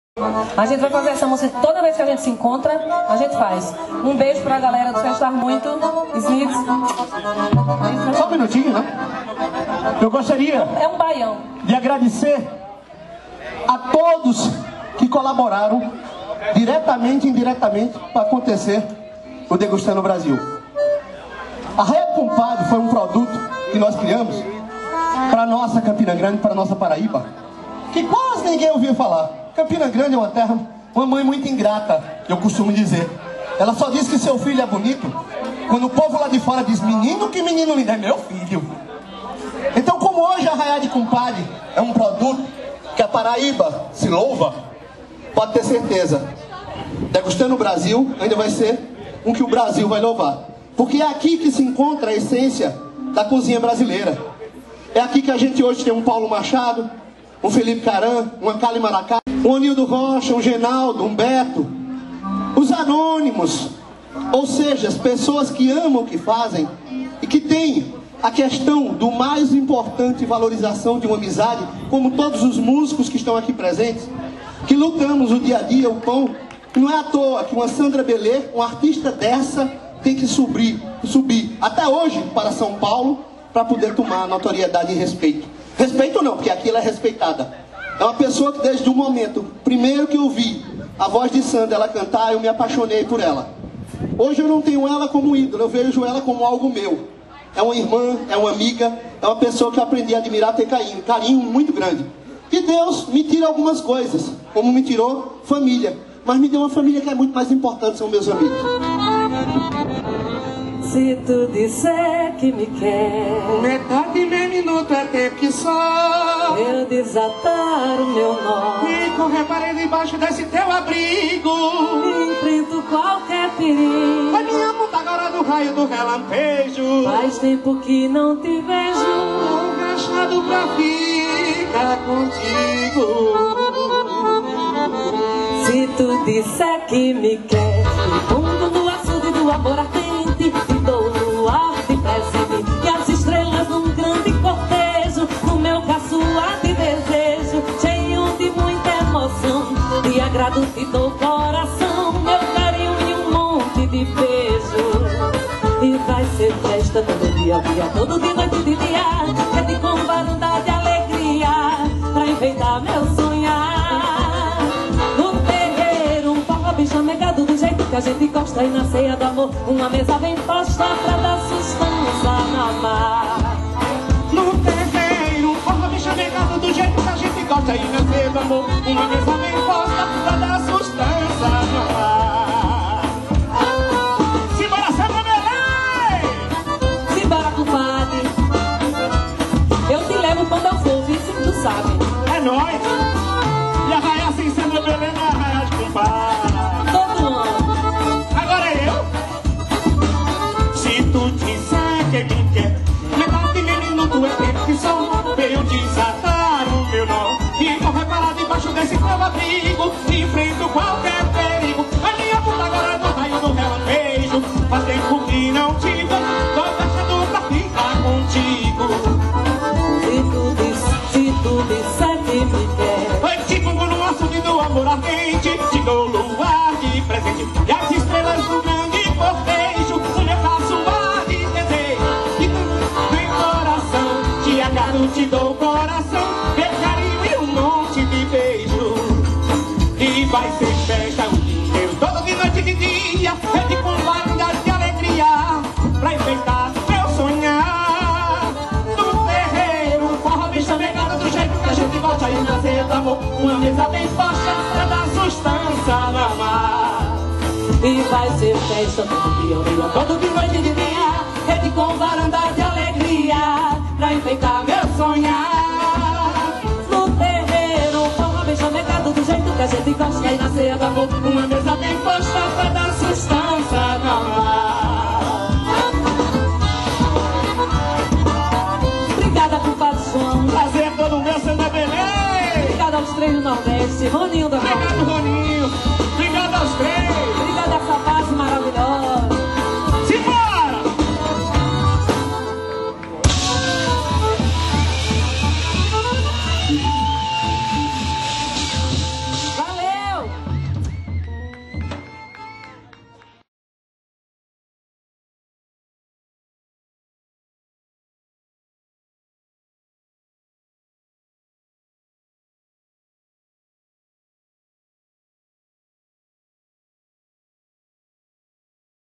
Sandra Belê e Cumpade João - Degustando Brasil
O encontro de Sandra Belê e o Cumpade João, no Festival Gastronômico Degustando Brasil, na Casa de Cumpade, distrito de Galante, em Campina Grande. Outubro/2017